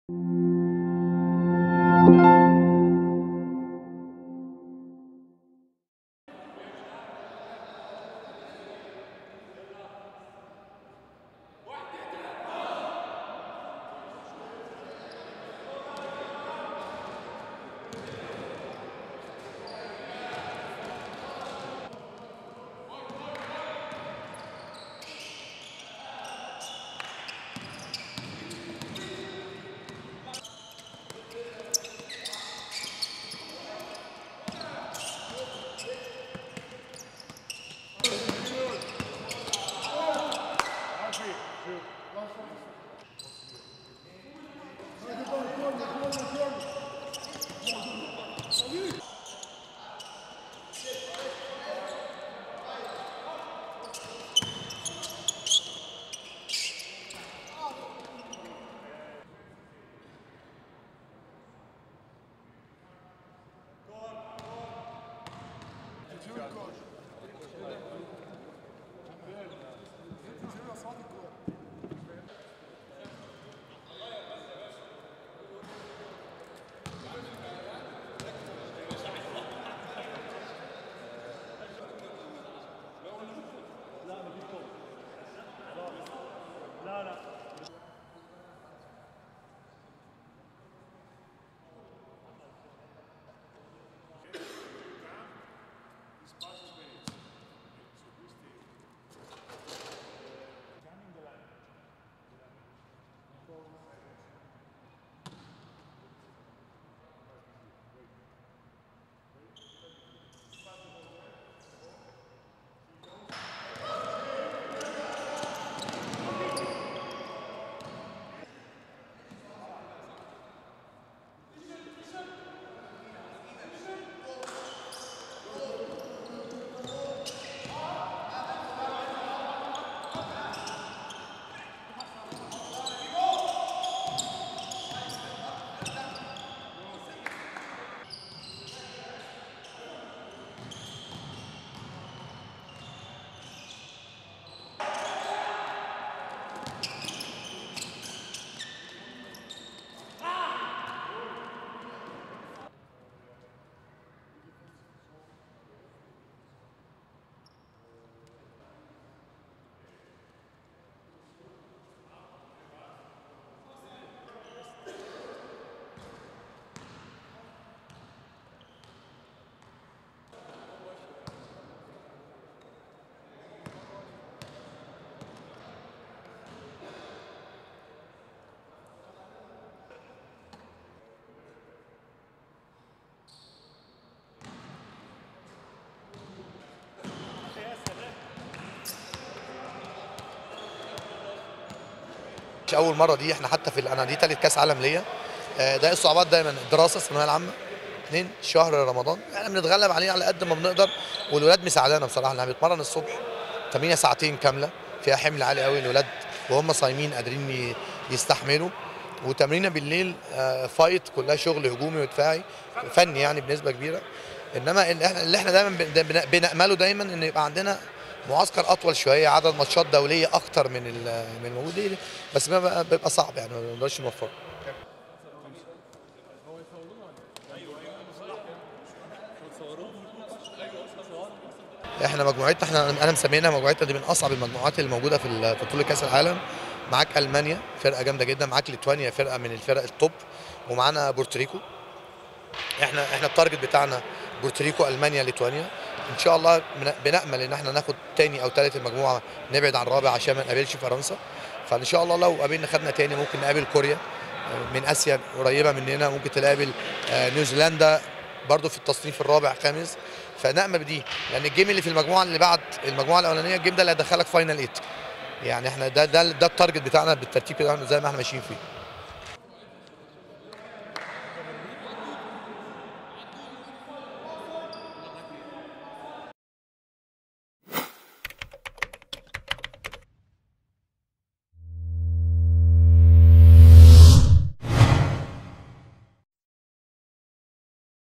EGYPT || Hosts Egypt gear up for Under-19 FIBA World Cup
SHOT LIST:

1. EGYPTIAN BASKETBALL PLAYERS HUDDLE HANDS AHEAD OF TRAINING SESSION
2. VARIOUS OF EGYPTIAN BASKETBALL PLAYERS DURING TRAINING SESSION
3. CLOSE OF BASKETBALLS IN A CARRY NET
4. WIDE OF PLAYERS DRESSED IN EGYPTIAN NATIONAL TEAM HOME KIT
5. VICE PRESIDENT OF EGYPTIAN BASKETBALL ASSOCIATION, MOHAMED ABDEL MOTELB, SPEAKS TO GROUP OF PLAYERS
6. BASKETBALL SPINNING UP ON THE FLOOR / PLAYERS AT BACKGROUND
7. VARIOUS OF COACH GIVES INSTRUCTION TO THE PLAYERS
8. MORE OF TRAINING SESSION
9. COACH GIVES INSTRUCTIONS
10. VARIOUS OF PLAYERS DISMISSING TO A BREAK
11.  CLOSE OF THE BASKETBALL HOOP
12. VARIOUS OF PLAYERS SITTING ON BENCHES
13. (SOUNDBITE)(Arabic) ASSISTANT COACH OF THE EGYPTIAN U-19 NATIONAL TEAM, TAREK KHAIRY, SAYING:
“This is my third World Cup appearance and the difficulties are the same: High school studies plus (the Muslim holy month of) Ramadan. We do our best to overcome that and the lads are helping us a lot because we hold morning training sessions that last for two hours while they are fasting, We also hold evening sessions in which we focus on the technical aspects of the game. We just hope we can have a longer training camp and play more friendly games. But this is always difficult”
14. PLAYERS COLLECT BASKETBALLS INTO A NET
15. (SOUNDBITE)(Arabic) ASSISTANT COACH OF THE EGYPTIAN U-19 NATIONAL TEAM, TAREK KHAIRY, SAYING:
"Our group is one of the most difficult at the tournament. We will play against a very strong team in Germany, a top team in Lithuania and Puerto Rico, Our target is: Puerto Rico, Germany and Lithuania. We hope we can finish either second or third in the group, not fourth because in this case we will face France.
“If we finish second, we may face Korea which is an Asian team that is close to our style of play or New Zealand, which is seeded fourth or fifth. The first knock-out game is crucial because it qualifies for the quarter-final, and our target is reaching the last eight”
16. WIDE OF PLAYERS

SCRIPT: Hosts Egypt are gearing up for the Under-19 Basketball World Cup as the team seeks to leave its mark in the prestigious tournament after some disappointing outcomes in its previous five appearances.
The Pharaohs are holding a training camp that is closed to the public to avoid any distractions ahead of the biennial showpiece that will take place from 1-9 July at the Cairo Stadium Indoor Halls Complex.
With Egypt becoming the first African country to host the U-19 World Cup, the national team is hoping it can make the most of home advantage and finish in a satisfactory position.
Egypt’s best position was two 11th-place finishes in the 2009 and 2015 editions.
To improve on that, they will need to sneak through in a tough group that includes Germany, Lithuania and Puerto Rico.
“Our group is one of the most difficult at the tournament. We will play against a very strong team in Germany, a top team in Lithuania and Puerto Rico,” said Egypt’s assistant coach, Tarek Khairy.
“Our target is: Puerto Rico, Germany and Lithuania. We hope we can finish either second or third in the group, not fourth because in this case we will face France.
“If we finish second, we may face Korea which is an Asian team that is close to our style of play or New Zealand, which is seeded fourth or fifth. The first knock-out game is crucial because it qualifies for the quarter-final, and our target is reaching the last eight,” he added.
Egypt are managed by Spanish coach Juan Antonio Orenga, whose experience can be vital in turning around their fortunes.
Orenga took charge of Egypt’s senior and U-19 sides in September last year, two months after steering Spain to the FIBA U-20 European Championship.
He declined to speak to the press to focus on the task ahead of him. Egypt’s players followed suit.
Egypt’s assistant coach listed the problems his side is facing in their preparations, almost two weeks before the World Cup gets underway.
“This is my third World Cup appearance and the difficulties are the same: High school studies plus (the Muslim holy month of) Ramadan. We do our best to overcome that and the lads are helping us a lot because we hold morning training sessions that last for two hours while they are fasting,” Khairy said.
“We also hold evening sessions in which we focus on the technical aspects of the game. We just hope we can have a longer training camp and play more friendly games. But this is always difficult.”
Egypt open their campaign against Puerto Rico on July 1st before taking on Lithuania one day later. They will conclude the group stage against Germany on July 4th.